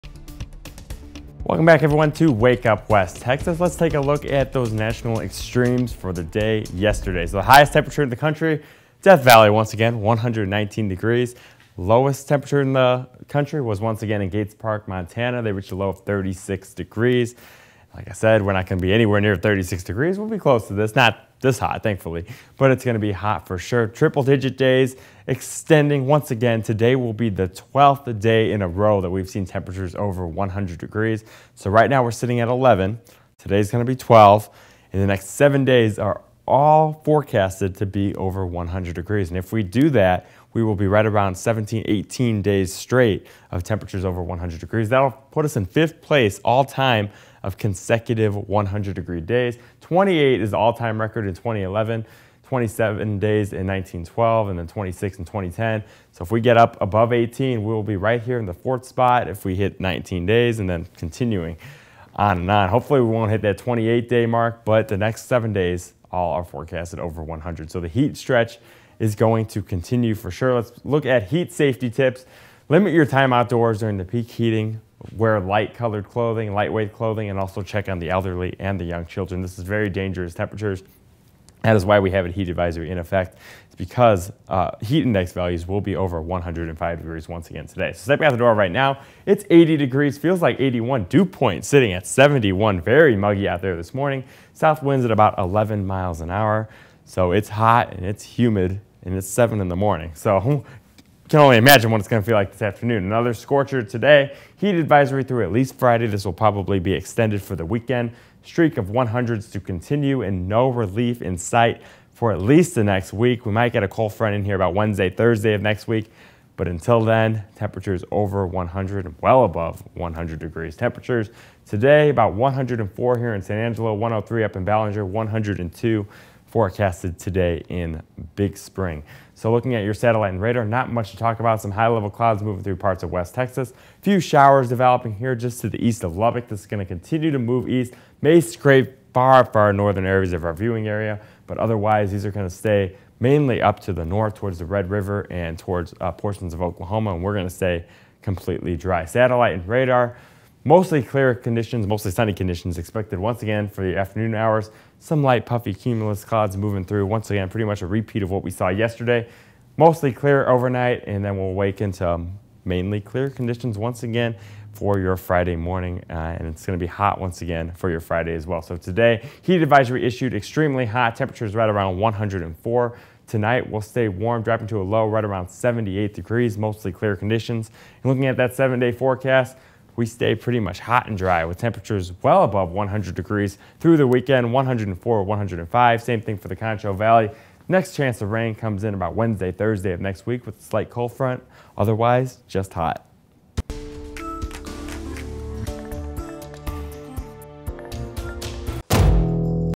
FORECAST: Streak of triple digits continue as West Texas bakes
Heat advisory once again in effect until at least Friday evening. This will likely be extended as the extreme heat looks to stay with us through the weekend.